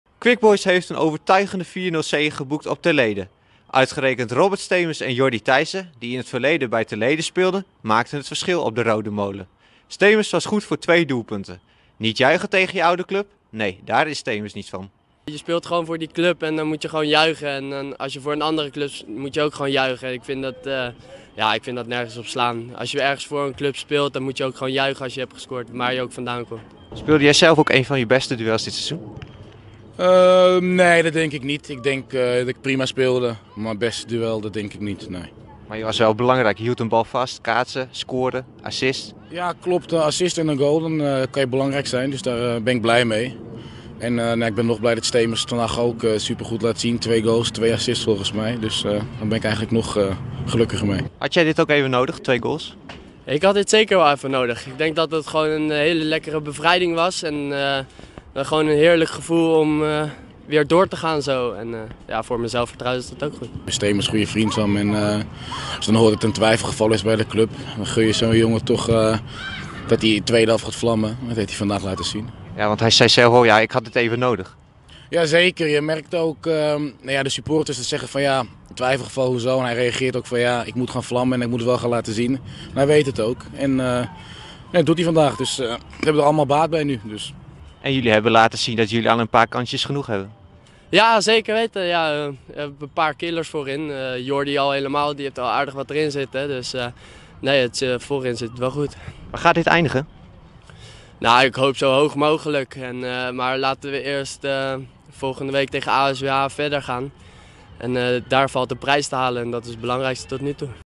'Je moet altijd juichen als je scoort tegen je oude club'
Quick Boys won zaterdag met 4-0 bij Ter Leede. Robert Steemers en Yordi Teijsse scoorden tegen hun oude club. En dat deed beiden deugd. Geen video over Quick Boys willen missen?